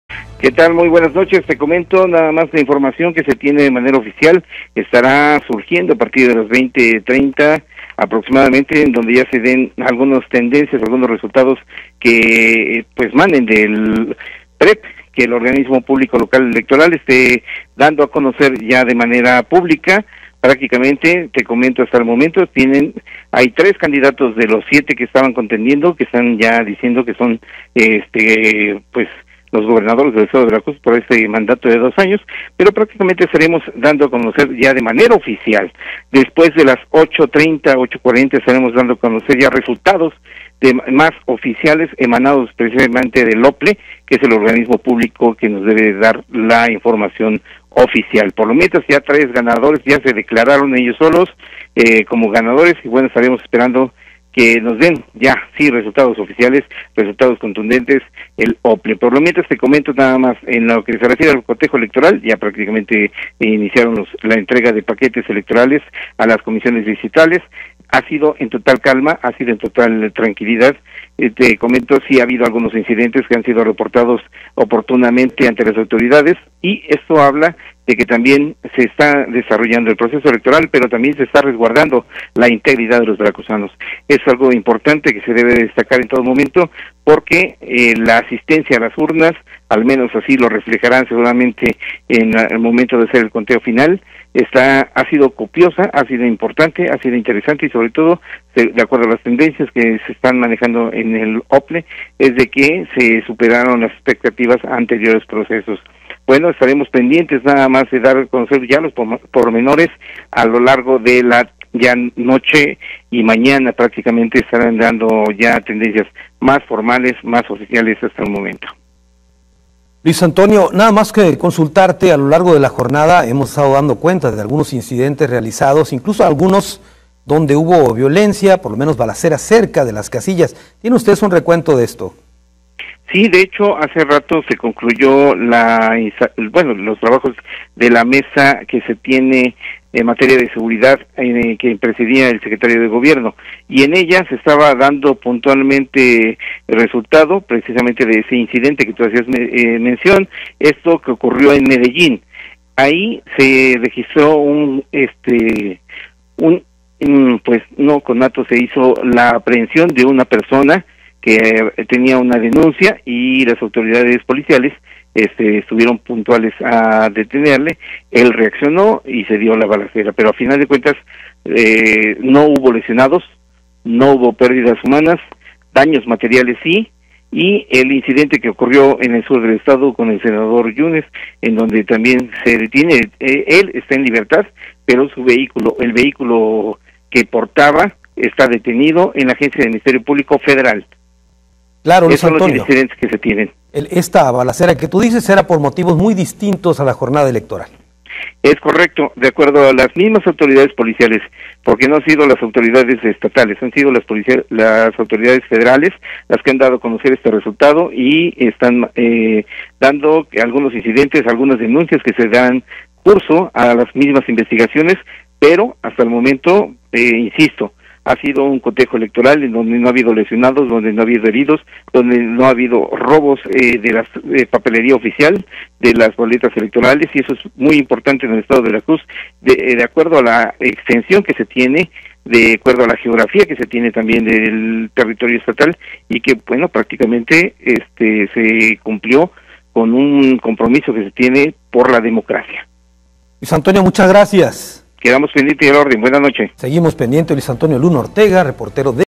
Veracruz no ha vivido una elección tranquila
Nos informa nuestro colaborador Luis Antonio Luna Ortega, desde el puerto.